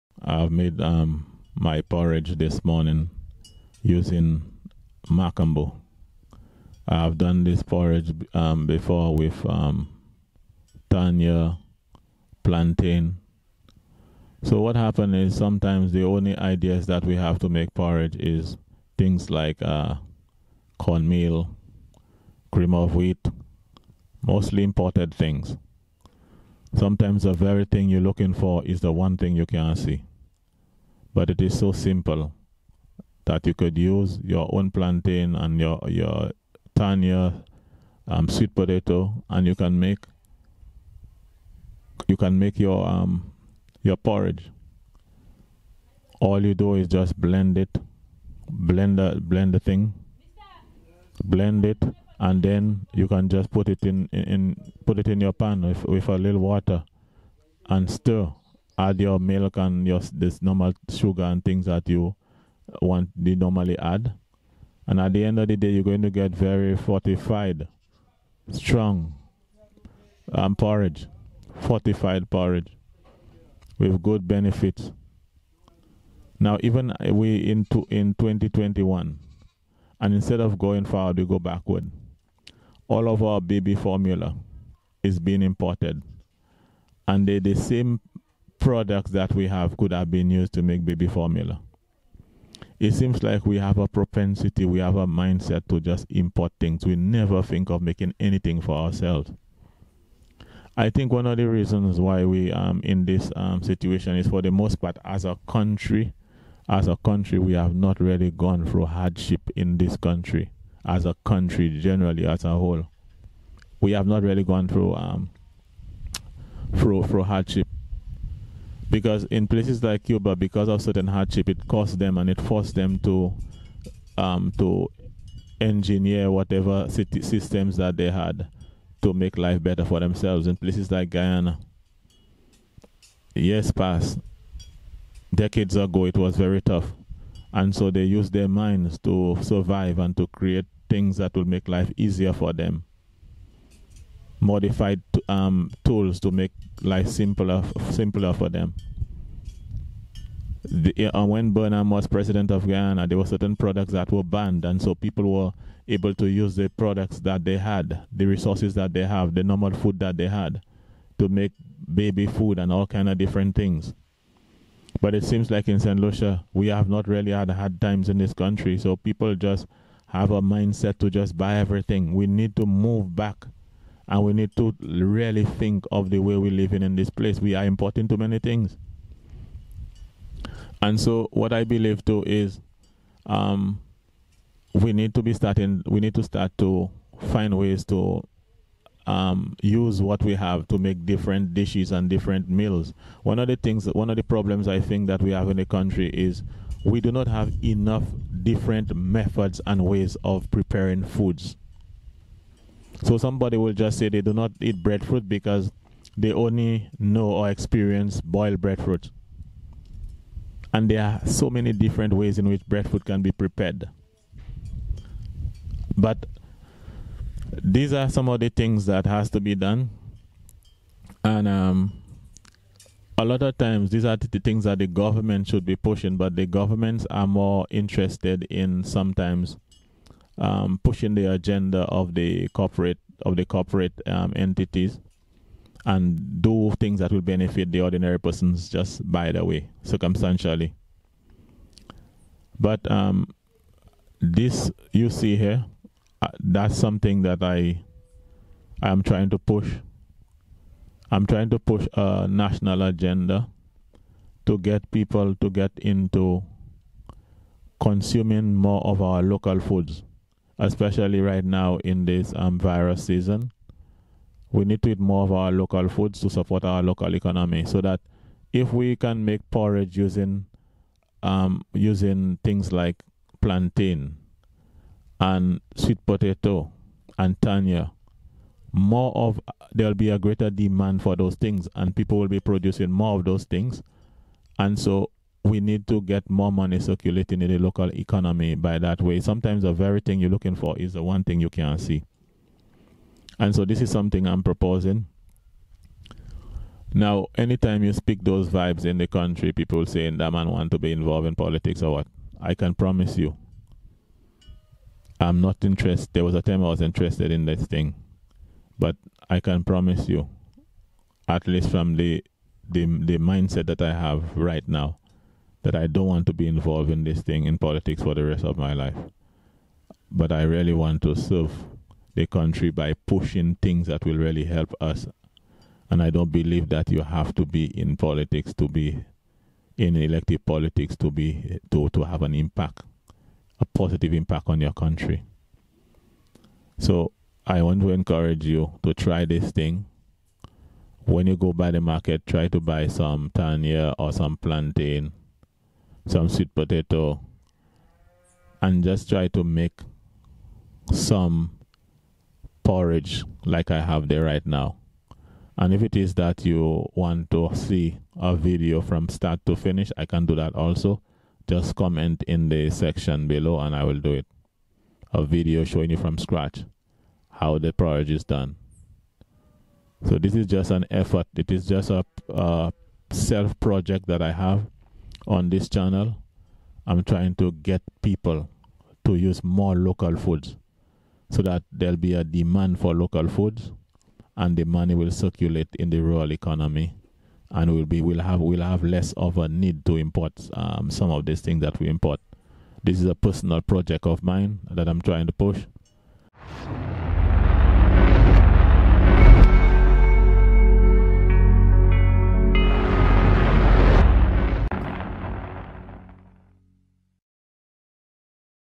HOW TO MAKE BABY FOOD LIKE THE OLD DAYS |MACAMBOO PORRIDGE
In this video we speak about how to make macamboo porridge as part of our national development strategy.